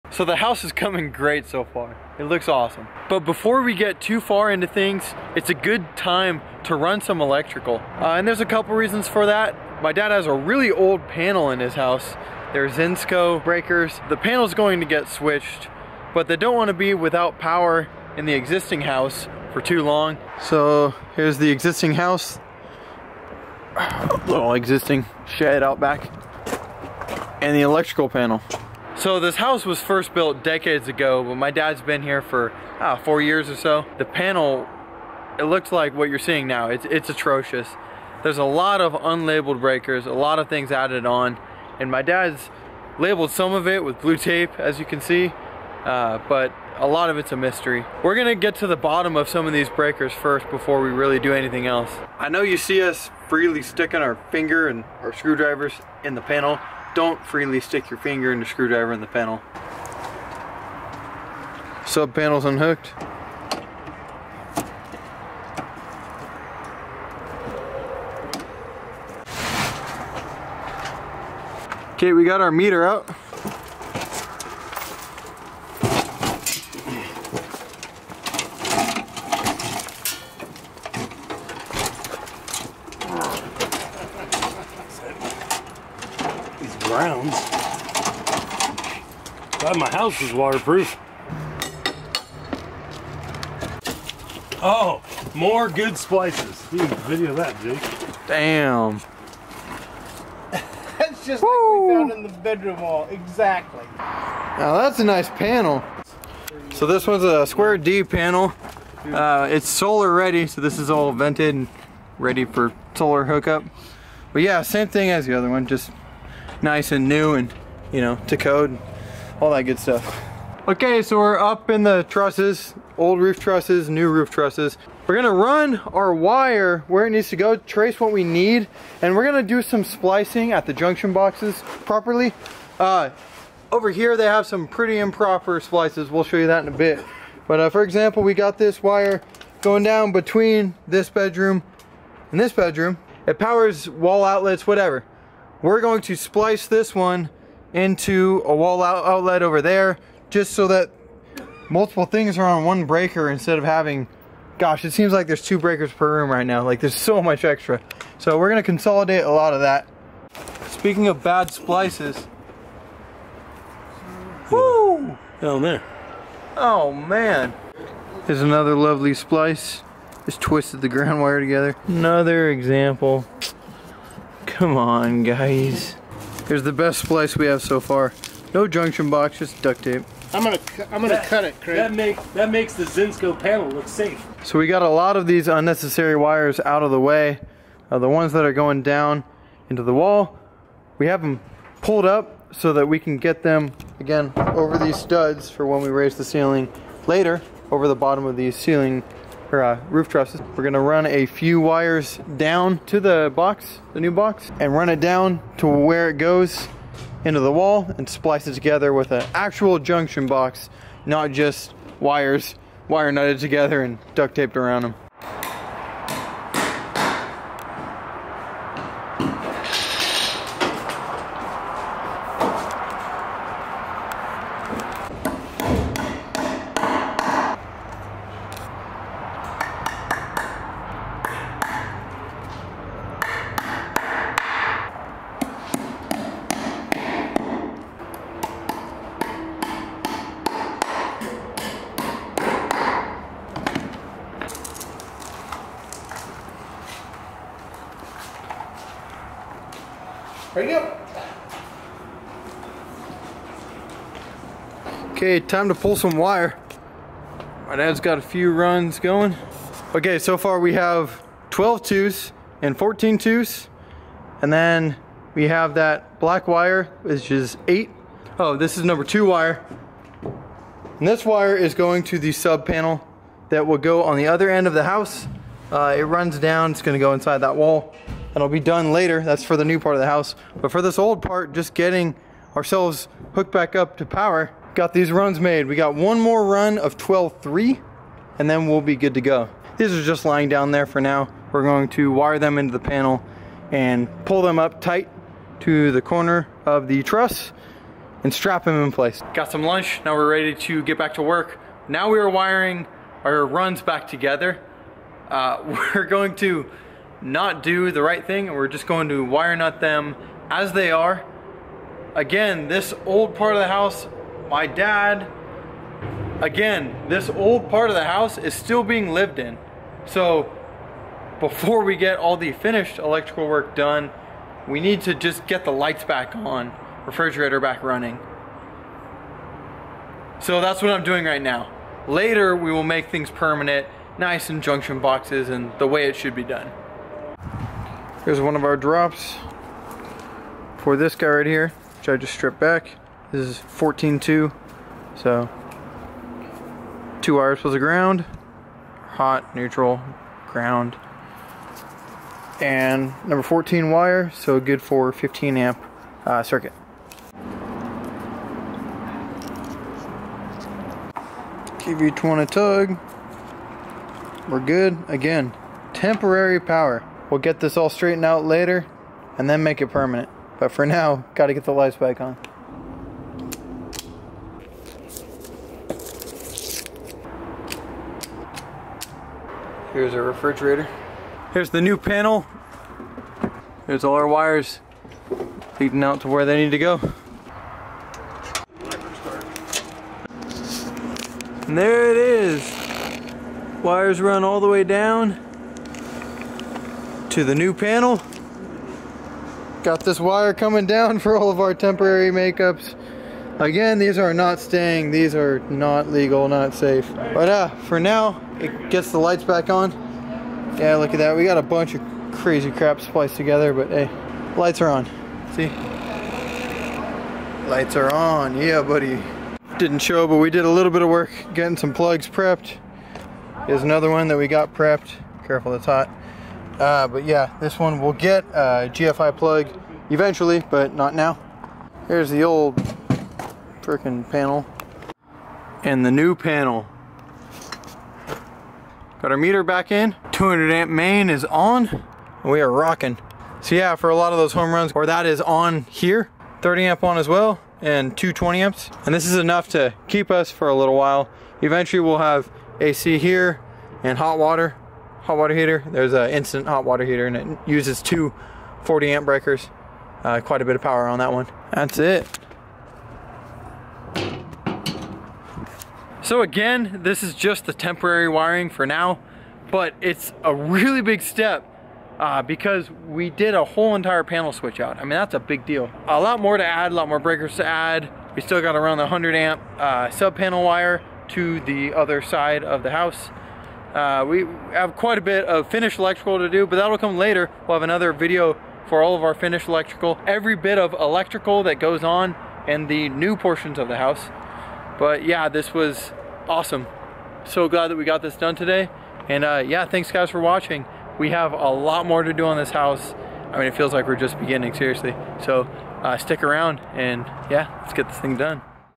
Huge Home Addition 7 - Electrical Panel Swap
Electrical time! We took a break from the rest of the build to swap the electrical panel before things got too sealed up.